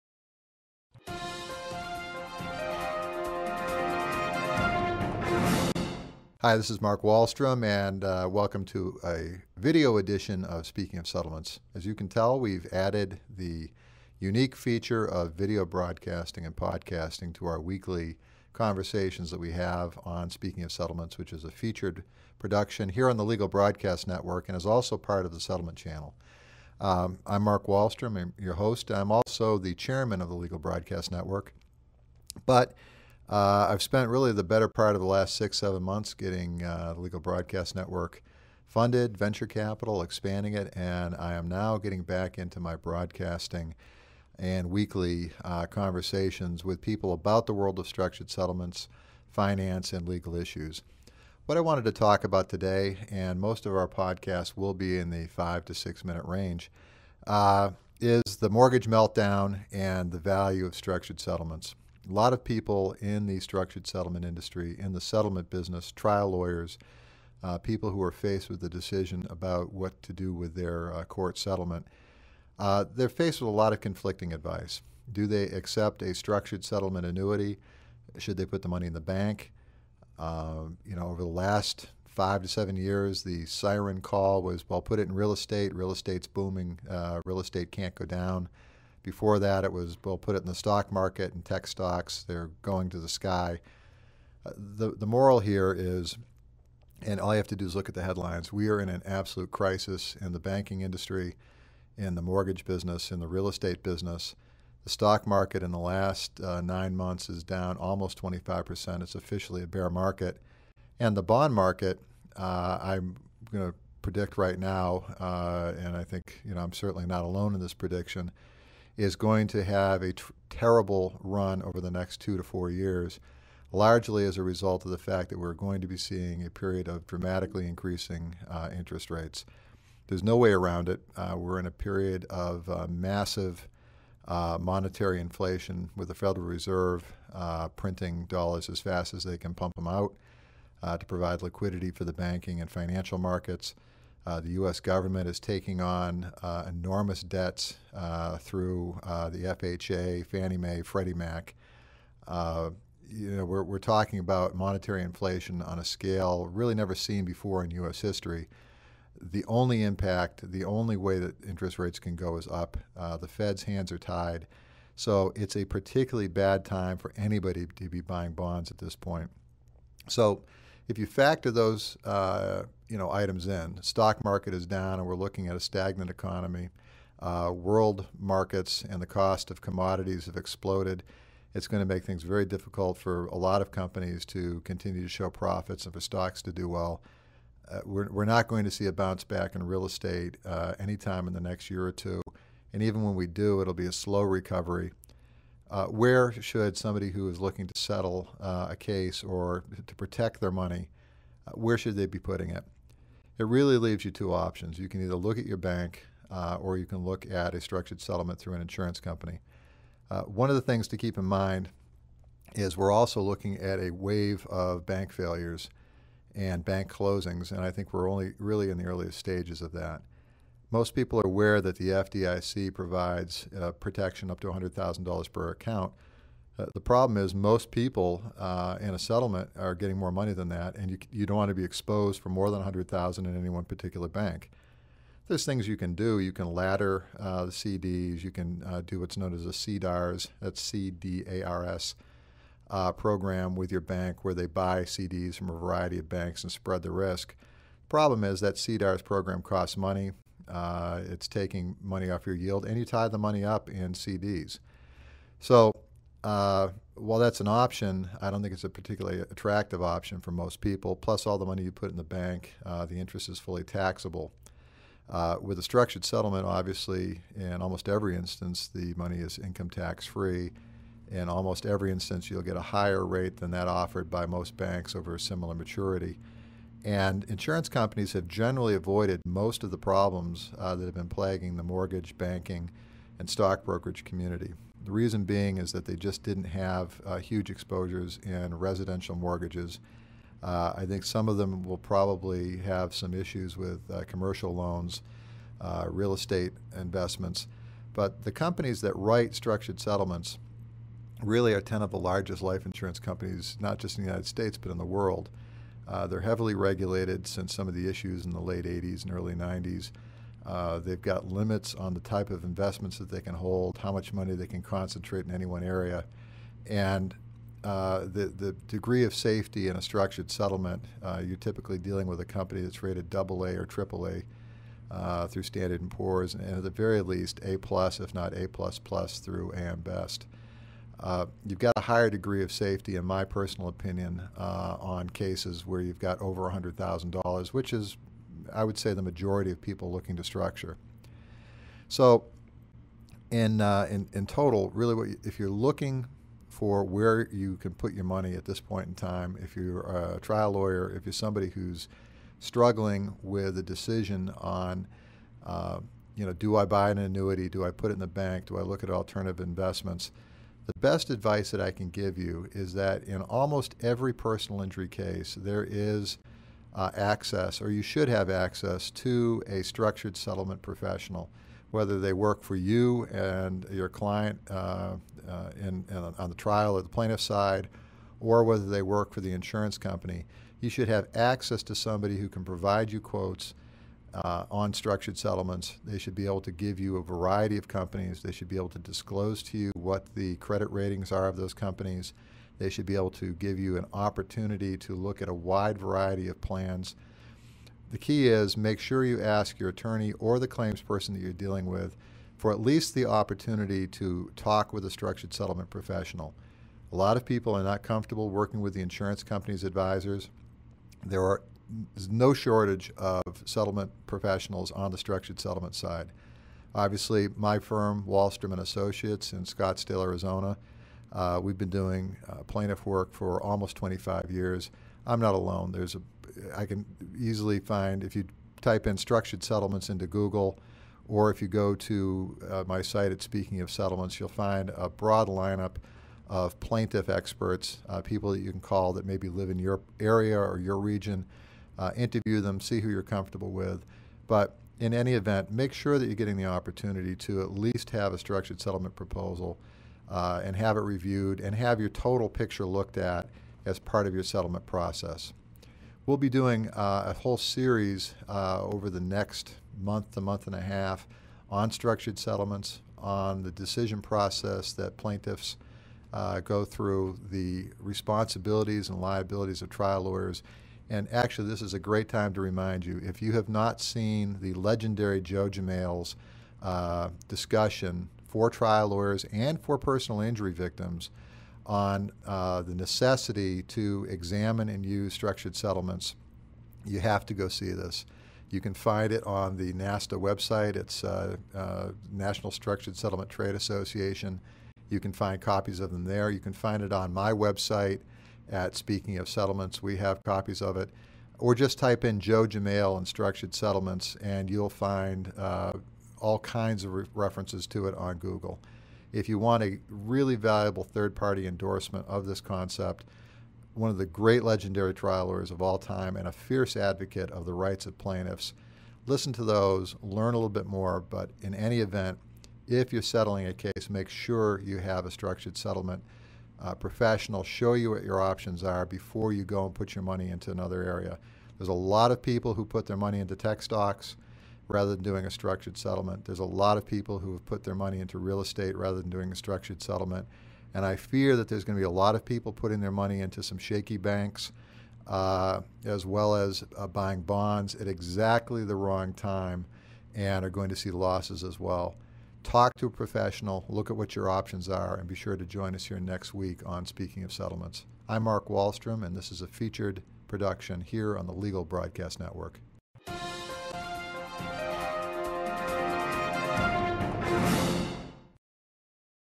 Structured settlements and the mortgage crisis
Mark Wahlstrom, Chairman of The Legal Broadcast Network and President of Wahlstrom of Associates presents his weekly commentary on Speaking of Settlements. This discussion is about the decision facing trial lawyers, plaintiffs and financial advisers as to the best choices for settlement dollars in this frightening market, particularly given the collapse of the mortgage market and it's impact on all other financial markets. What are the options you have, where should you put your money and why does a structured settlement annuity still stack up as the best option for a clients settlement dollars. If you are a trial lawyer or injury victim considering what to do with your funds, a structured settlement remains one of the single safest, secure and tax advantaged choices for the vast majority of cases.